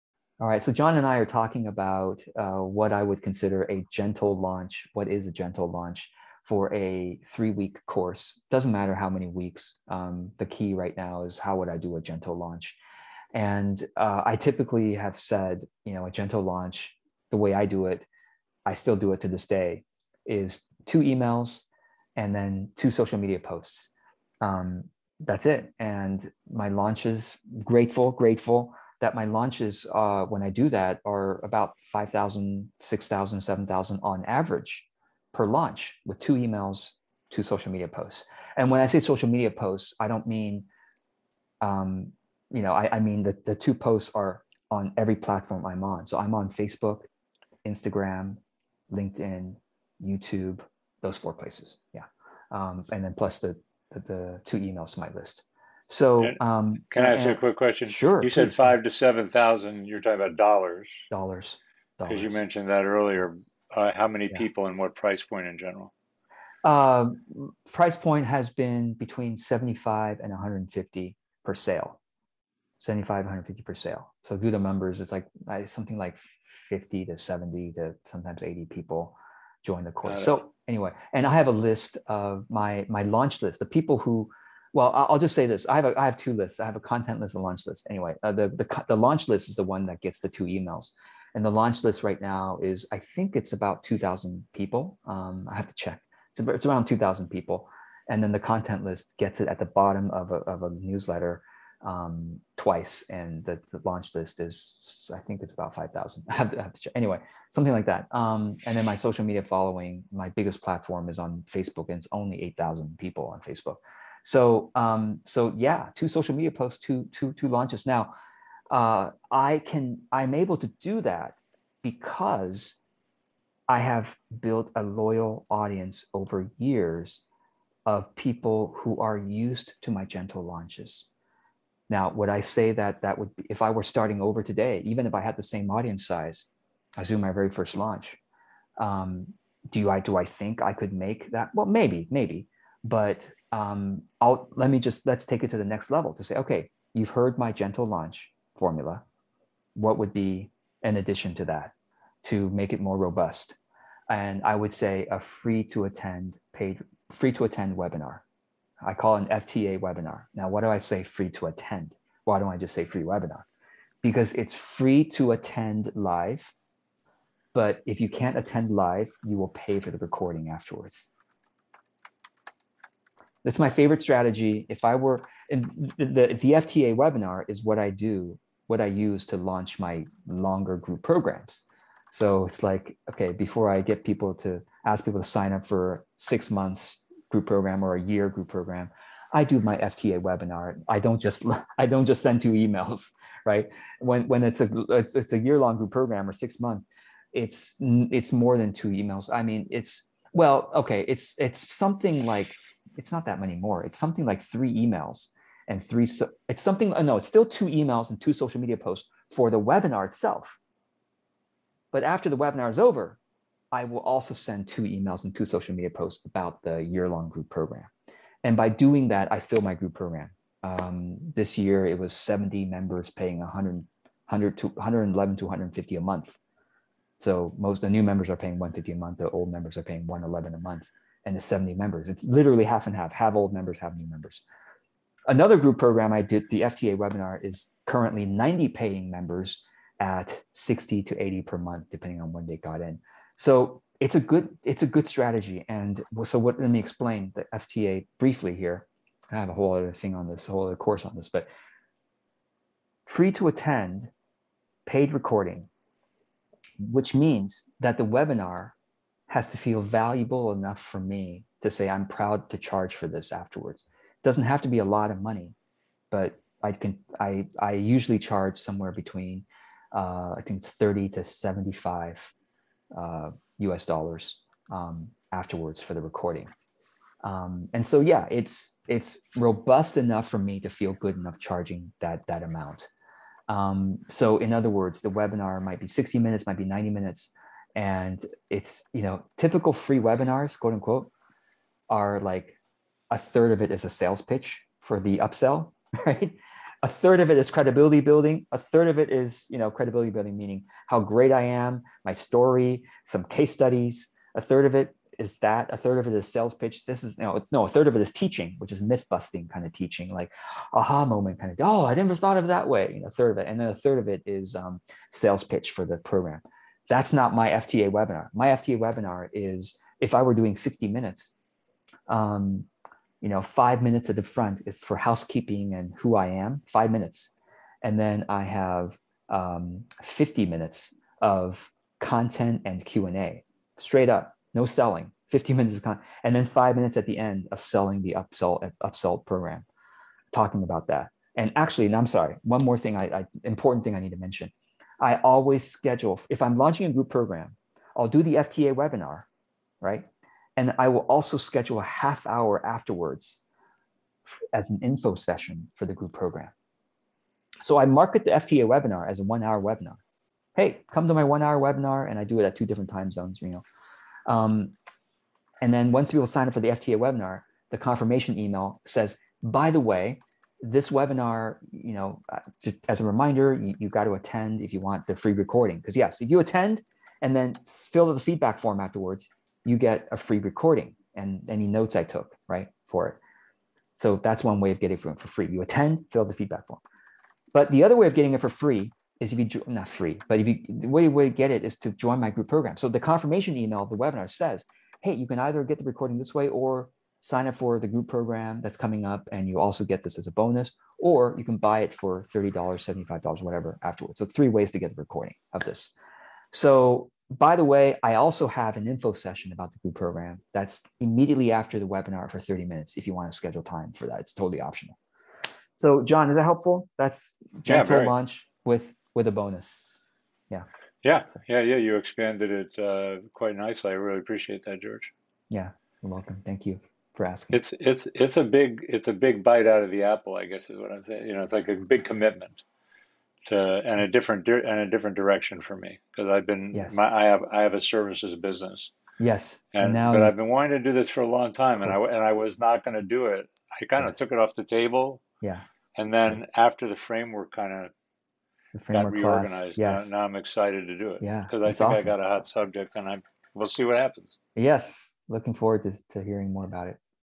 Gentle Launches: an overview
This is a segment from my Monthly Q&A call. You get access to 2 months of these live calls when you sign up for any of my courses

MonthlyQA